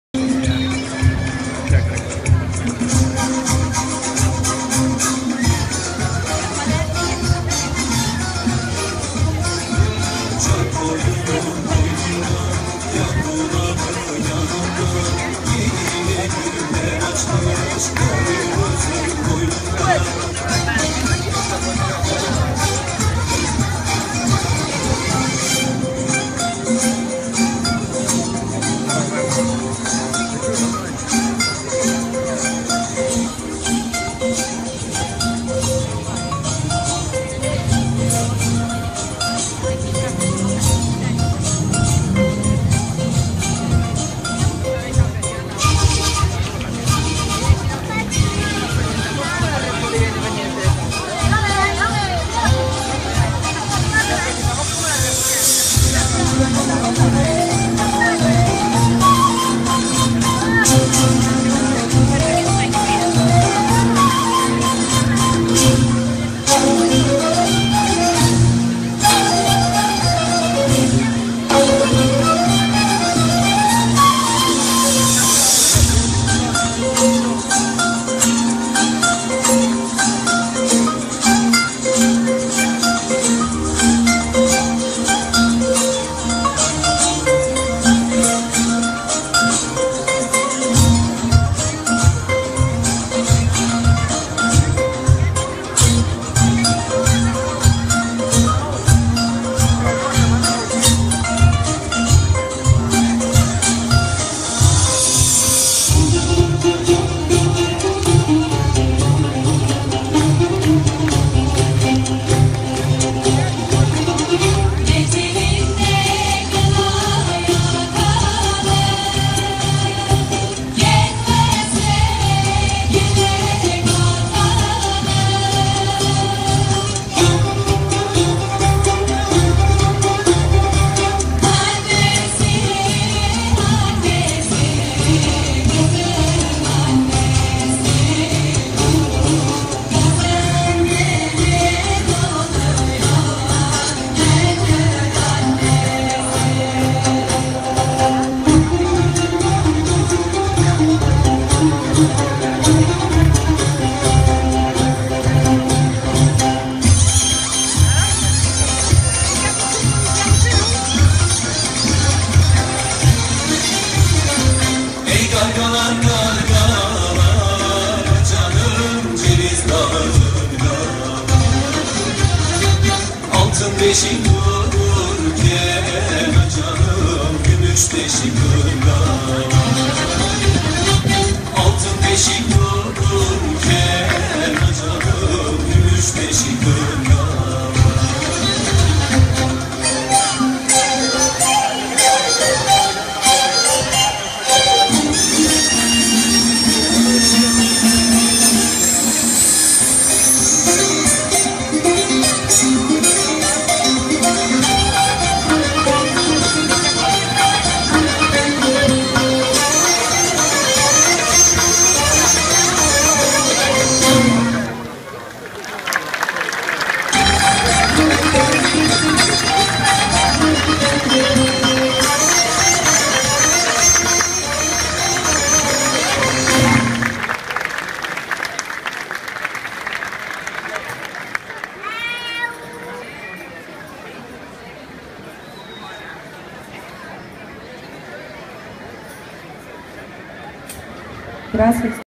Festival Internacional De Folclore Alegria 2015 - Turkish Dancers.
International festival of folk dancing 2015 in Calella, featuring performers from Turkey. Awesome!